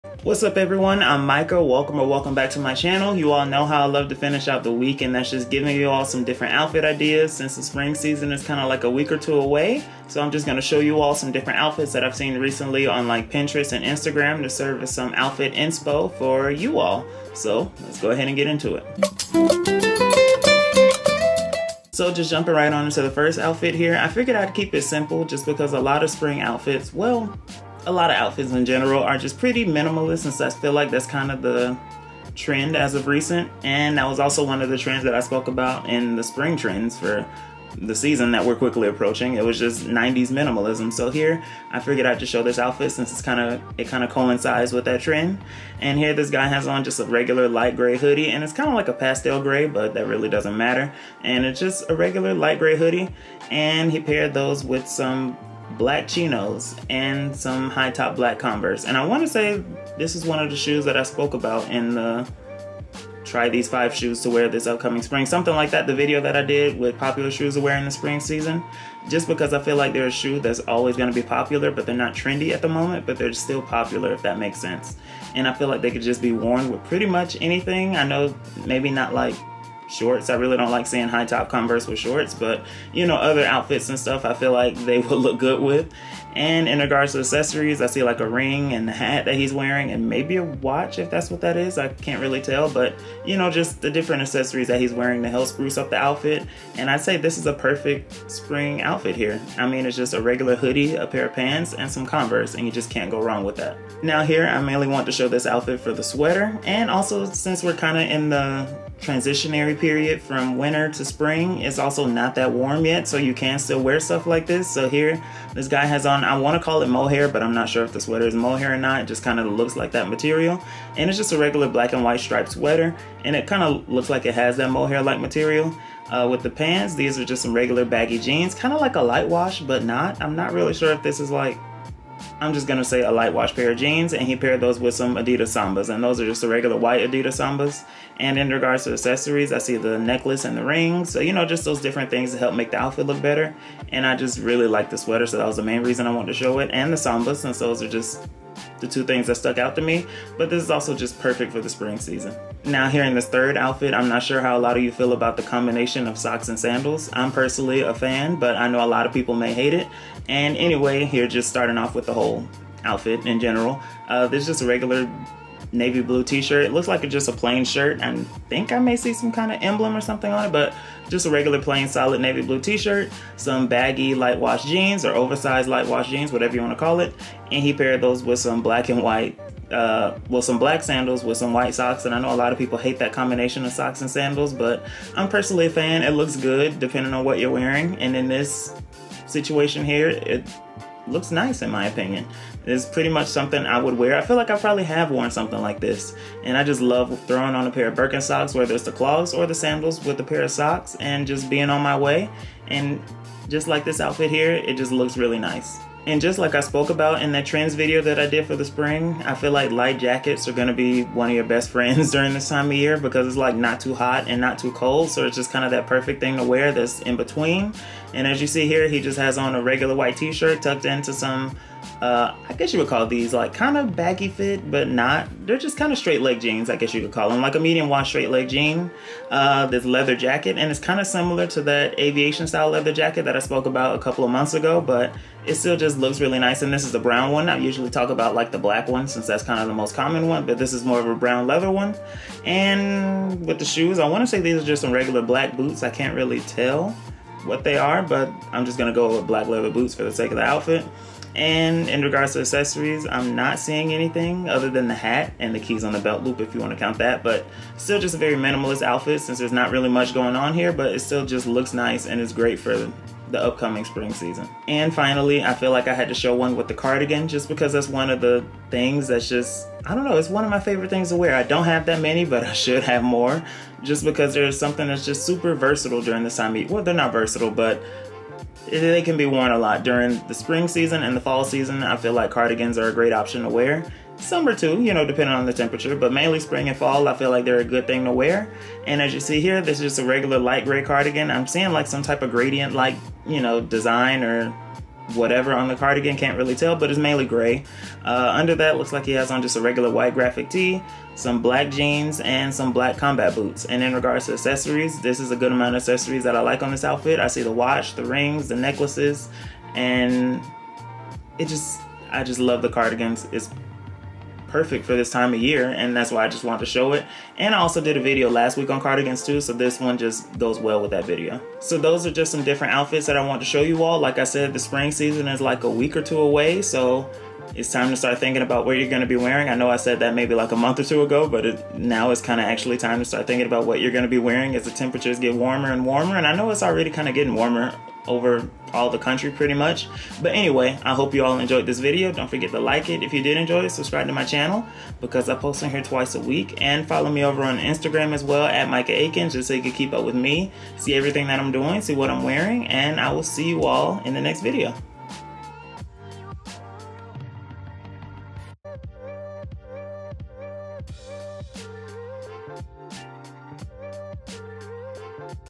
What To Wear This Spring | 5 Outfit Ideas | Men's Fashion Style Blog 2024
I get a lot of my style inspiration by looking at influencers on Pinterest and Instagram, and then tweaking their styles to fit my personal style. It allows me to see how different trends are interpreted by different people, and gives me an unlimited amount of ideas. Here in this YouTube video, I show you all some outfit ideas from Pinterest.

Join the familia!

Chapters:
Intro: 0:00
Outfit 1: 0:40
Outfit 2: 1:43
Outfit 3: 2:32
Outfit 4: 3:25
Outfit 5: 4:24
Outro: 5:21